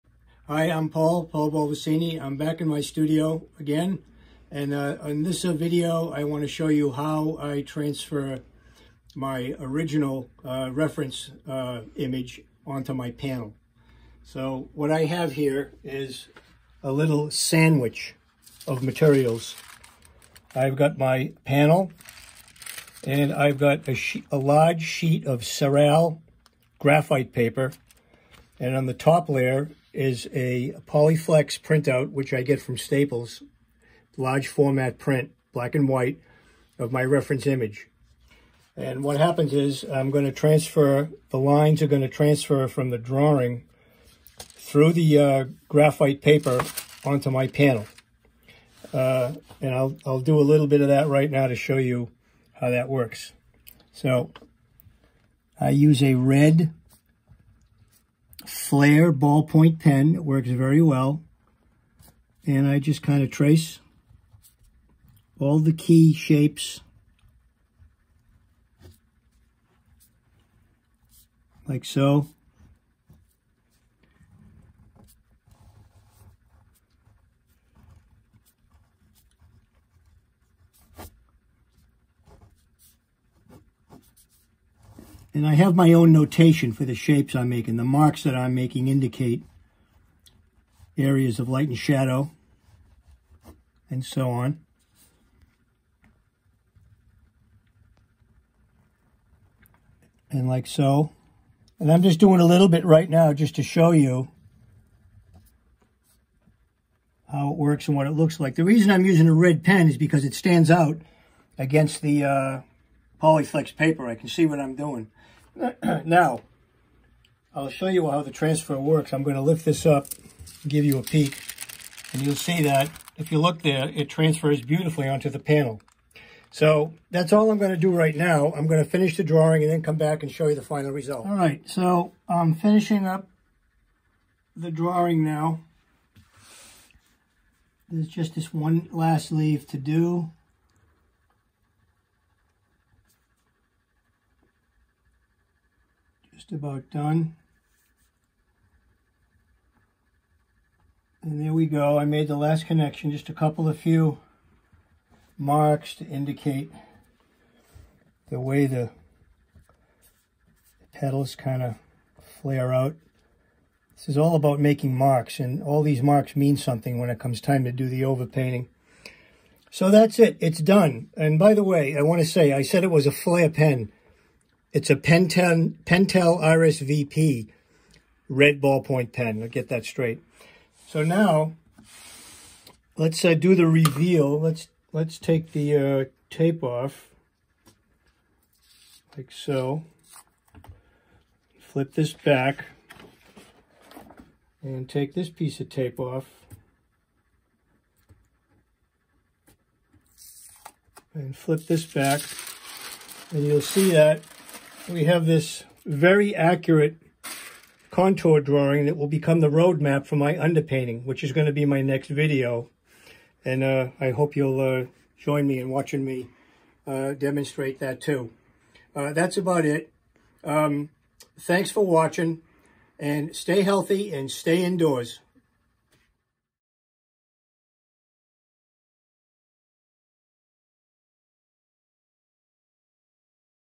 Graphite paper transfer of drawing onto a prepared panel
Artist Paul Baldassini demonstrates how to transfer the drawing from the source image on the prepared panel. The source image was created from the working color file in Adobe Photoshop using Calculations and Curves to produce an acceptable grayscale image. A high-resolution file is then created and output onto heavyweight Polyflex paper at my local Staples. This is then trimmed and placed on top of a sheet of Saral Graphite Transfer Paper which is attached to a prepared panel forming a working “sandwich”.  The drawing is then transferred using a Pentel RSVP red ball point pen.